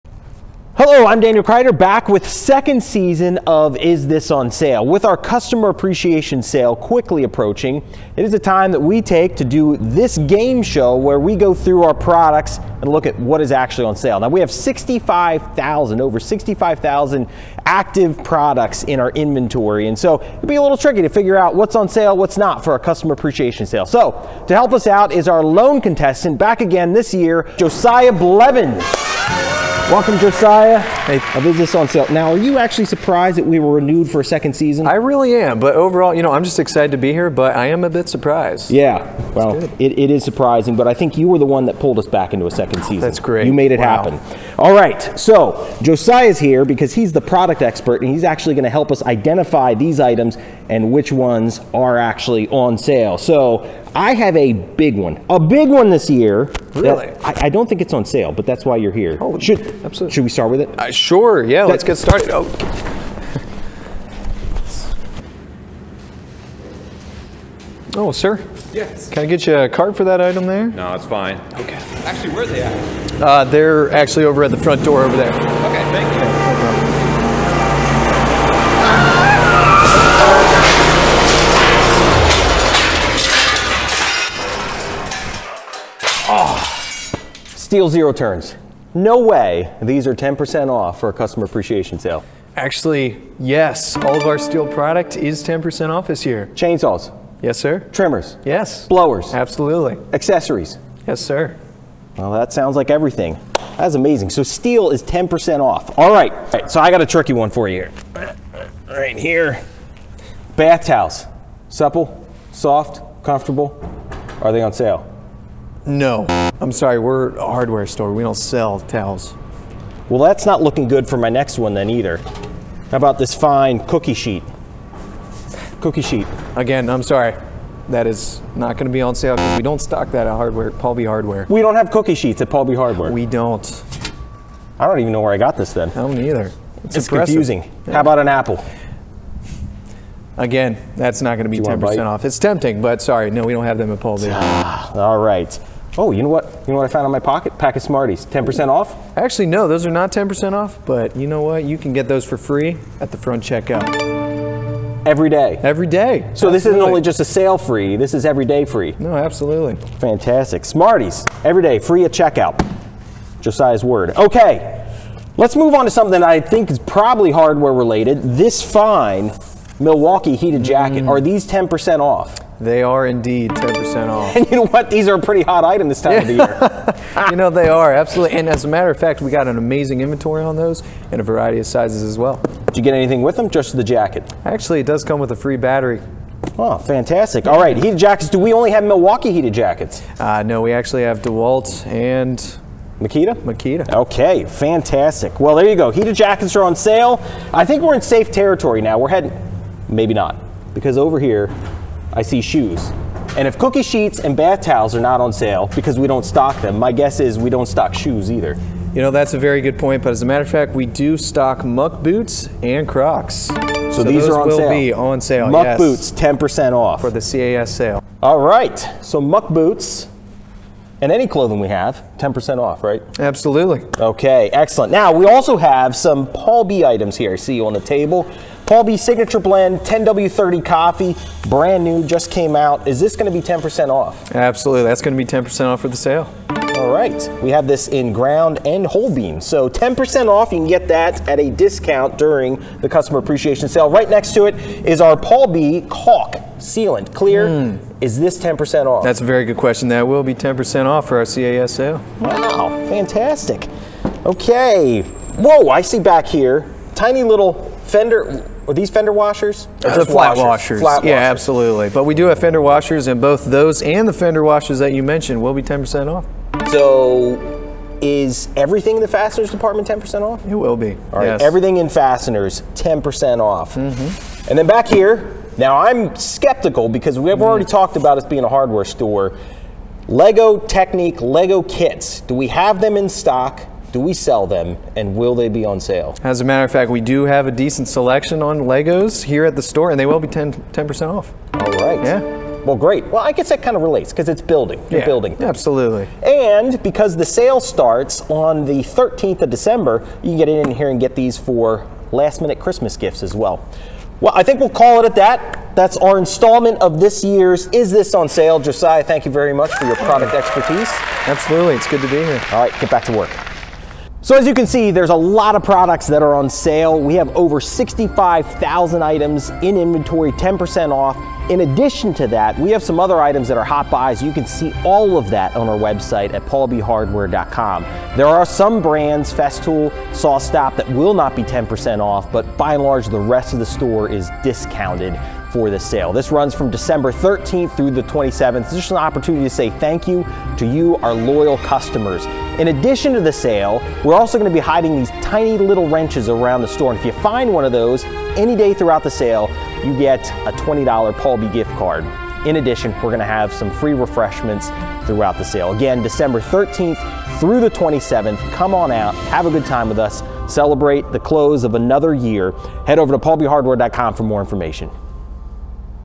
PaulB Hardware, store-wide 10% off sale!
Dan and Josiah are back with the second season of "Is this on Sale?" to help you navigate PaulB Hardware's Customer Appreciation Sale. What products does PaulB carry? Which ones are on sale? When is the sale? We'll answer all of your questions. If you still want more details, you can find them